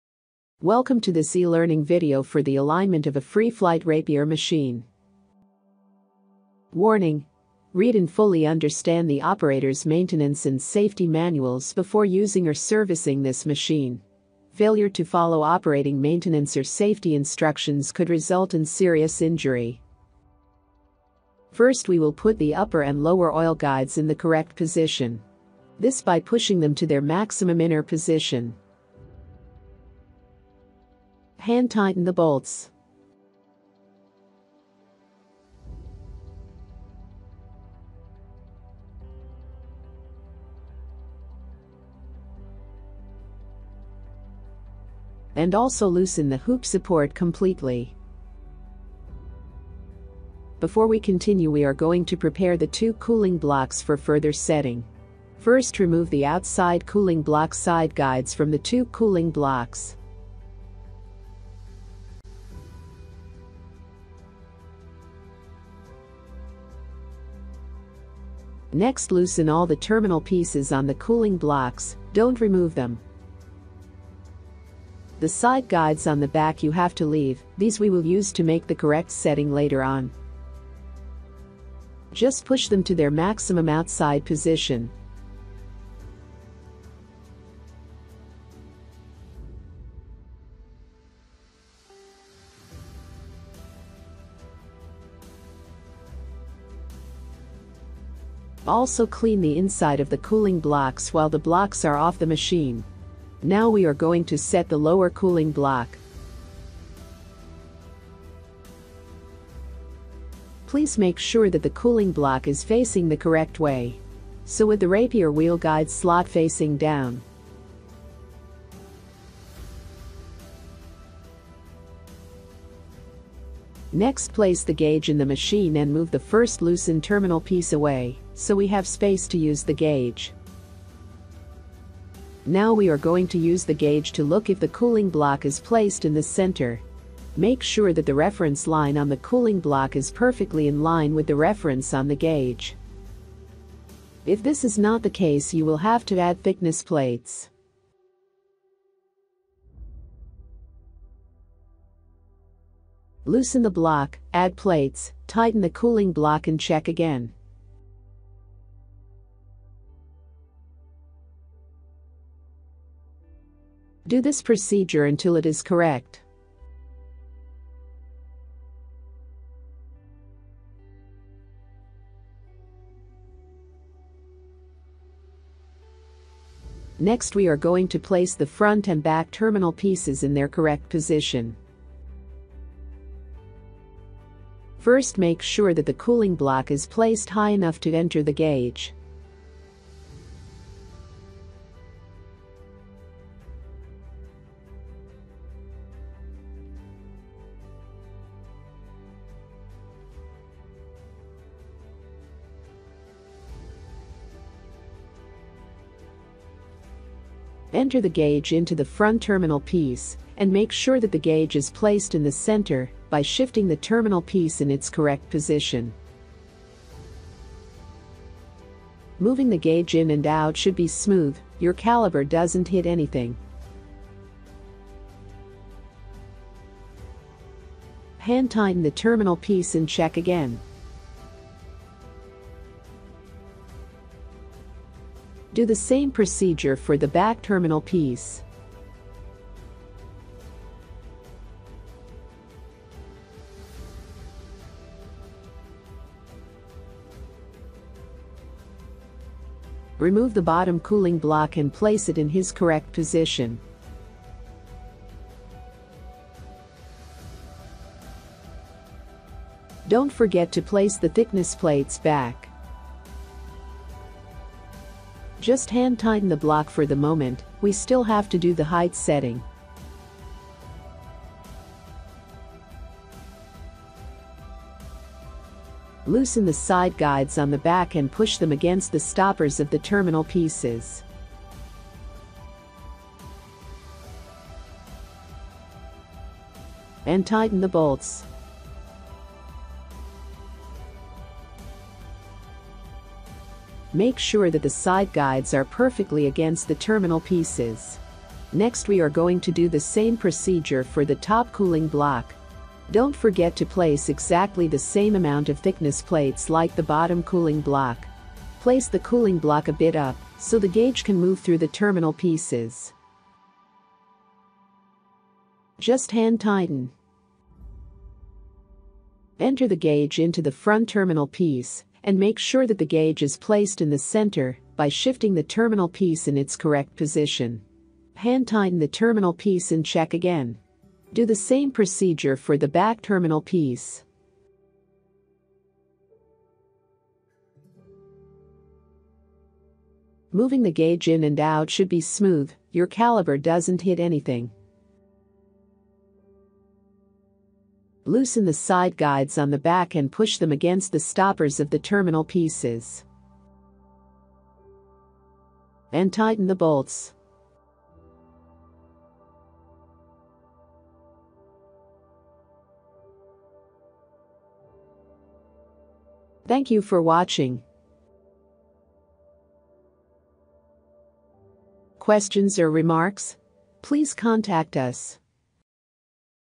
FF Alignment OptiMax-i part 5 of 6: Setting the side guides cooling blocks
In this video we guide you how to do the FF Alignment.
02102020